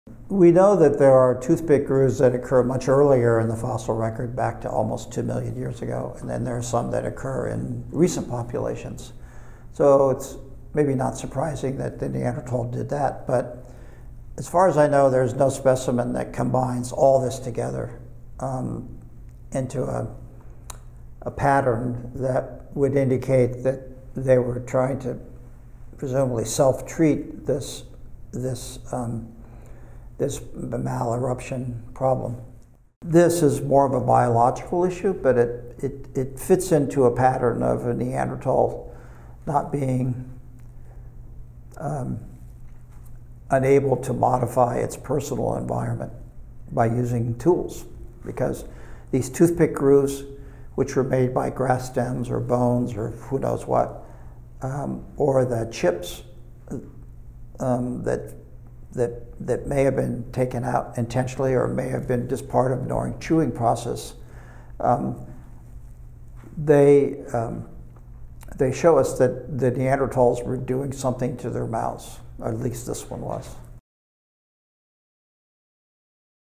Researchers find evidence of Neanderthal dentistry
David Frayer, professor emeritus of Anthropology at the University of Kansas, discusses a recent study he led that analyzed toothpick grooves and other signs of manipulation to four teeth recovered from the Neanderthal site of Krapina in Croatia. The analysis provides evidence of prehistoric dentistry to address tooth pain that took place 130,000 years ago.